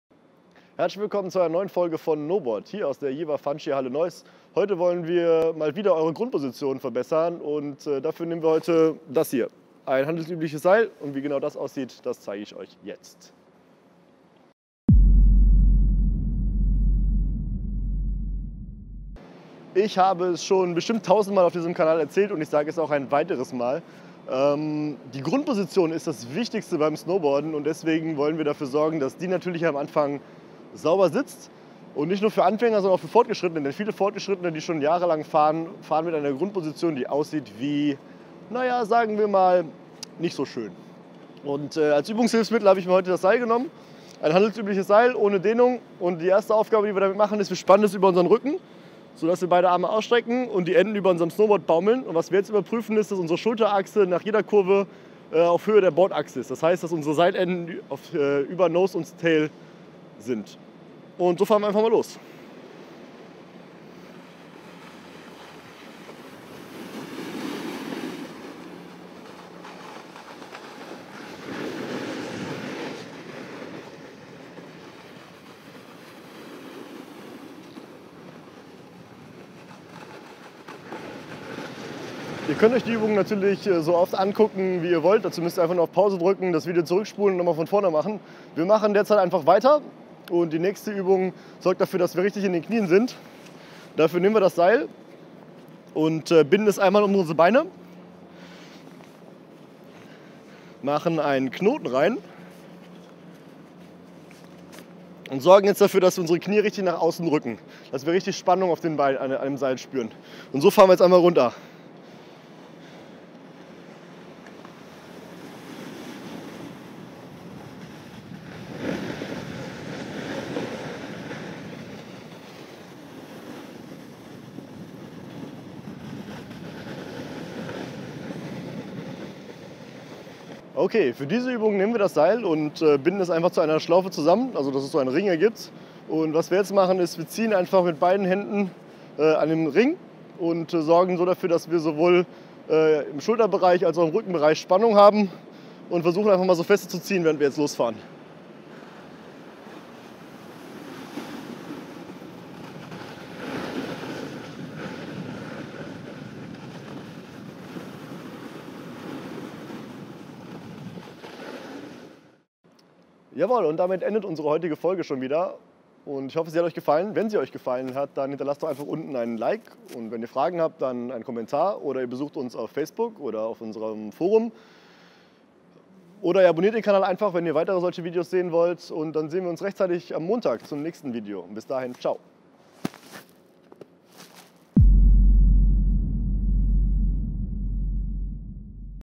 Übungen für Körperspannung in der Snowboard Grundhaltung
Die Grundhaltung ist das Wichtigste beim snowboarden. In dieser Folge wollen wir ein bisschen Körperspannung hinzufügen und dazu benutzen wir das Seil aus der letzten Folge.